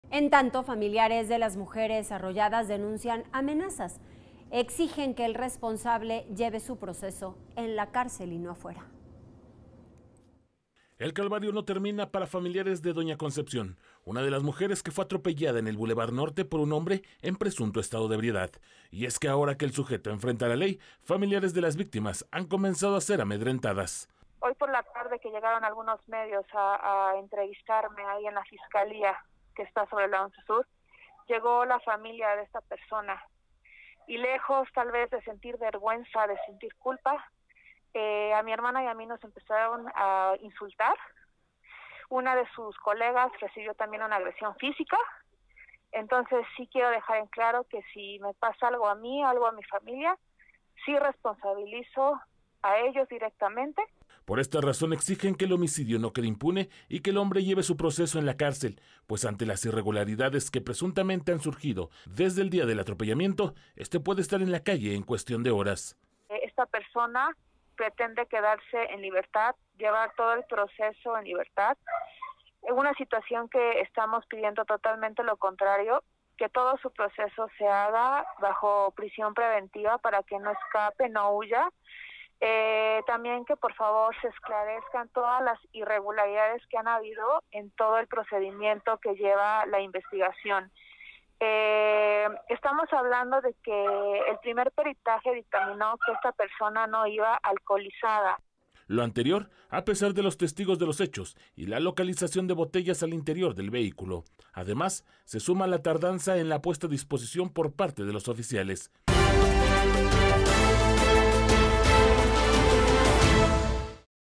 Amenazas e insultos a familiares de víctimas | Las Noticias Puebla
Familiares de las mujeres arrolladas denuncian amenazas y exigen que el responsable lleve su proceso en la cárcel y no afuera.